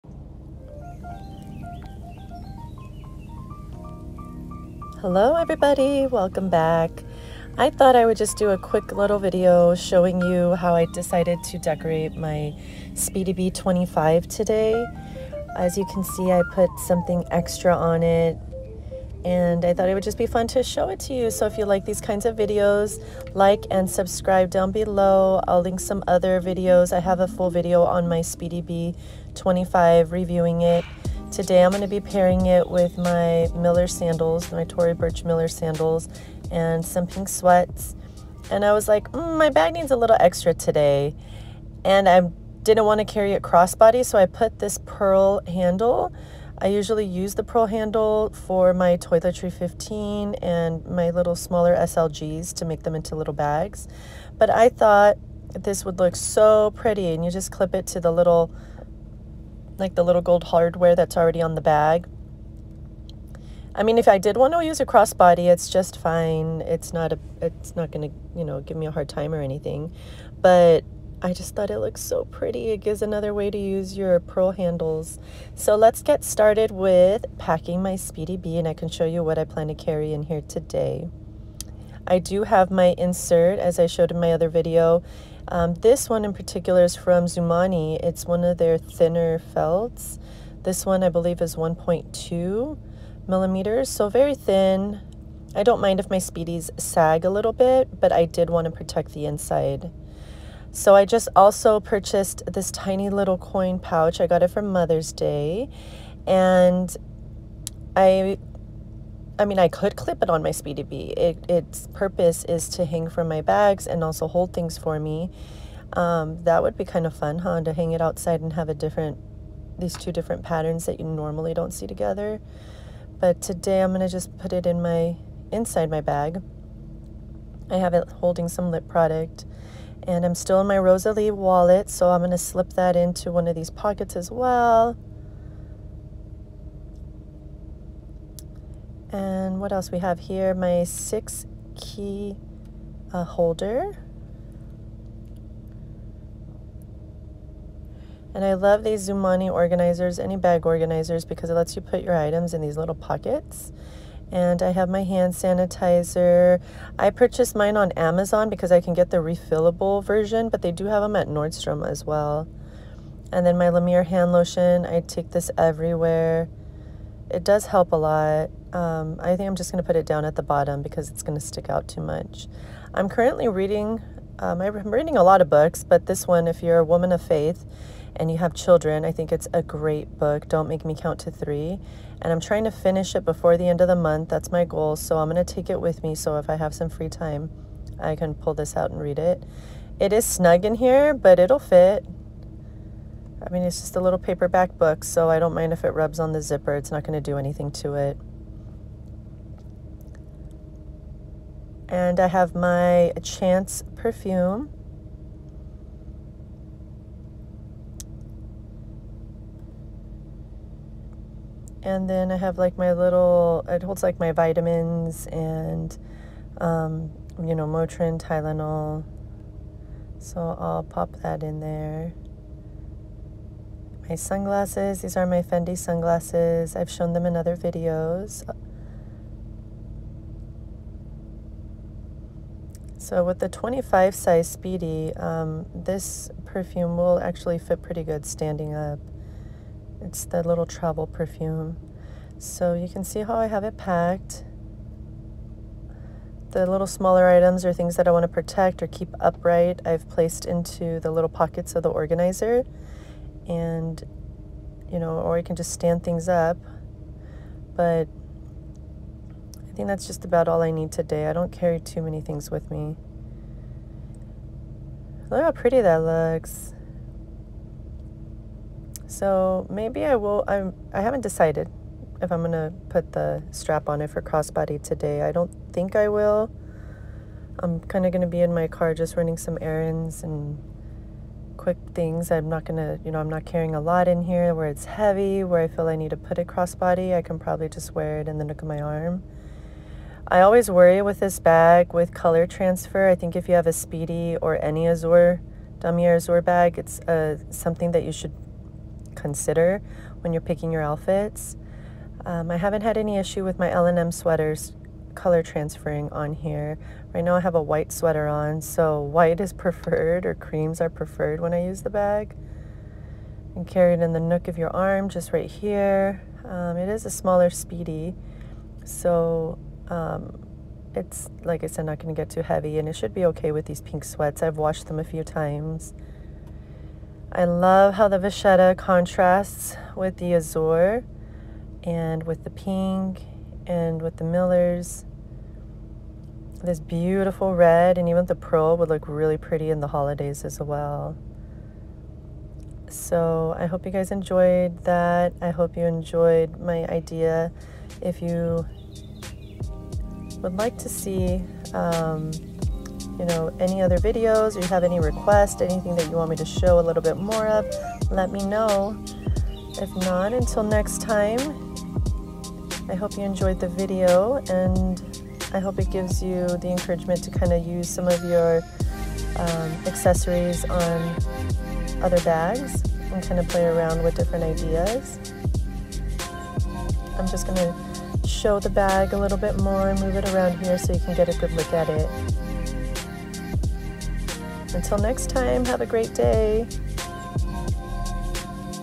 LV Speedy B 25 Damier Azur | Glow up | what’s in my bag | Samorga Pearls
LV Speedy B 25 | Glow up

I love all things LV, fashion, and Lux mommy. Life’s too short, let’s enjoy all things beautiful 🤍

Items shown in video:

LV Sppedy 25 Bandouliere: Speedy Bandoulière 25 Damier Azur - Women - Handbags | LOUIS VUITTON ®

Zoomoni bag organizer: Bag Organizer for Louis Vuitton Speedy 25 (Organizer Type B) - Zoomoni

🤍Samorga pearl Handel: (Pearl-4) Pearl Handle Strap – SAMORGA® Perfect